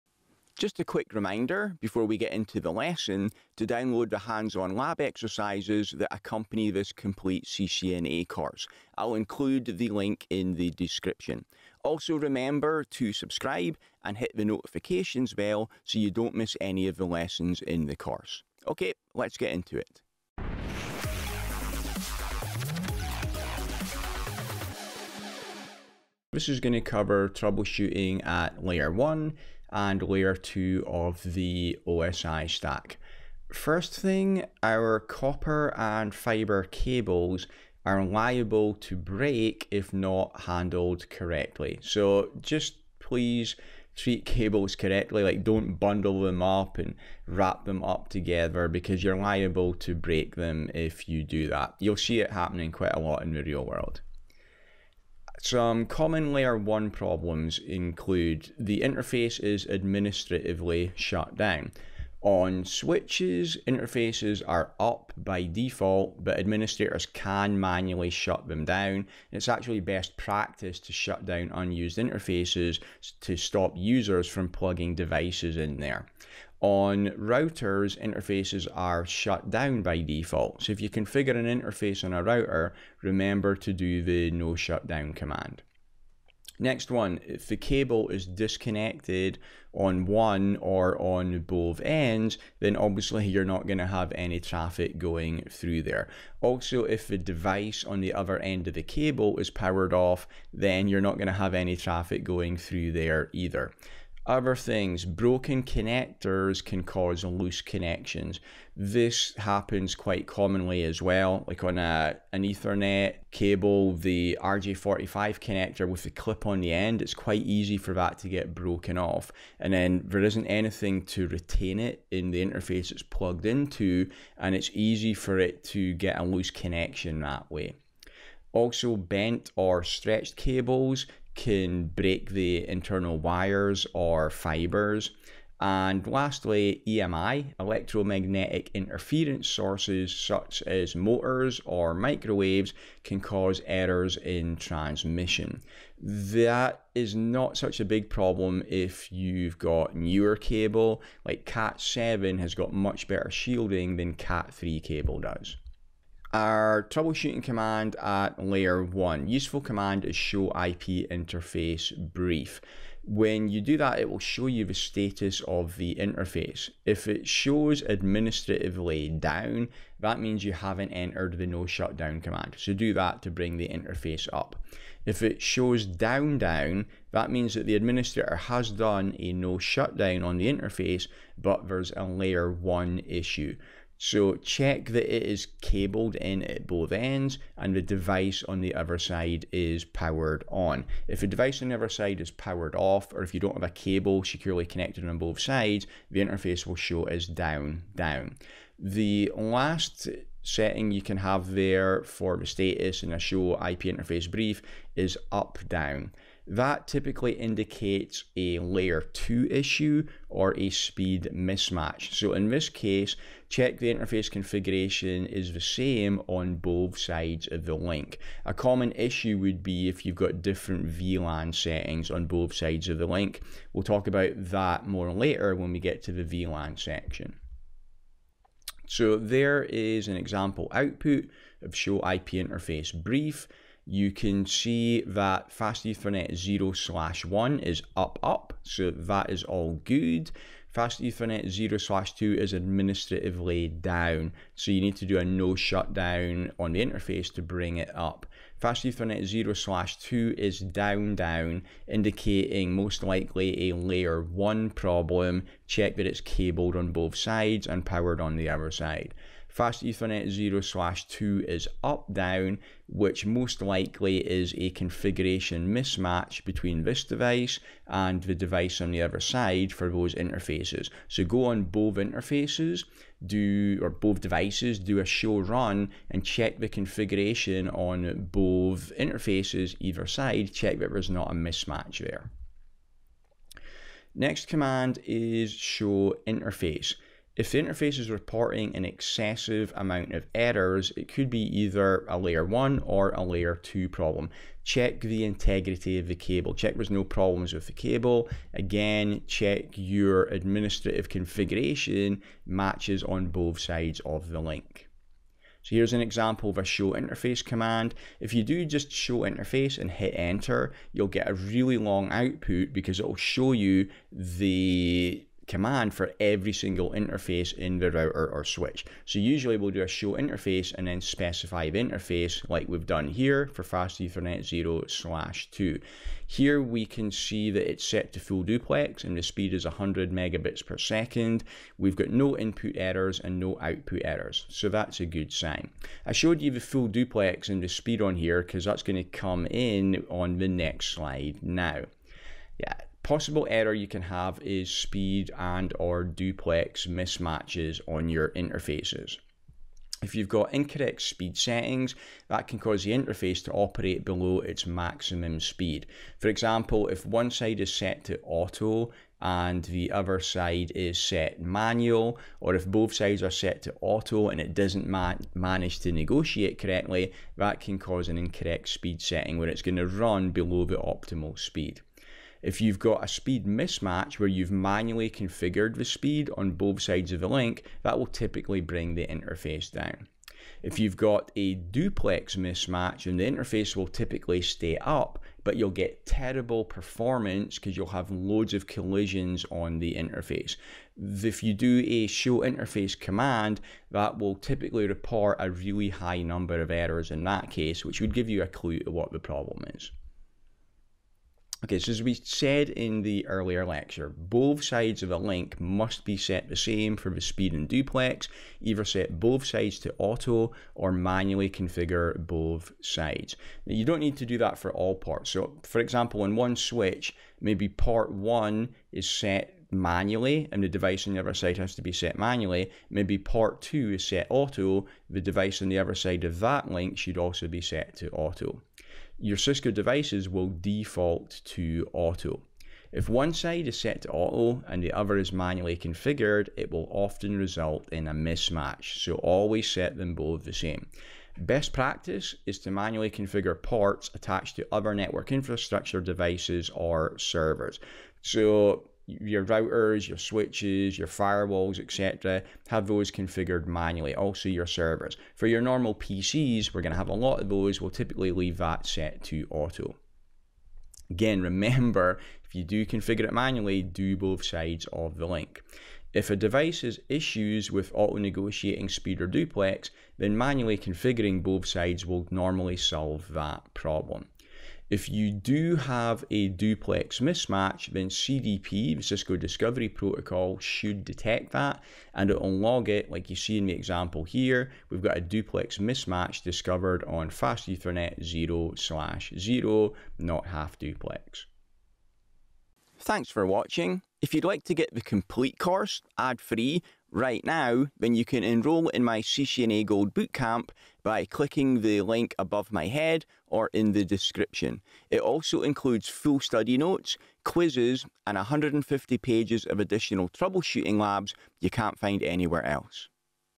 Free CCNA 200-301 Course 14-06: Basic Layer 1 and 2 Troubleshooting
I explain Basic Layer 1 and 2 Troubleshooting in this video.

Thanks!
Neil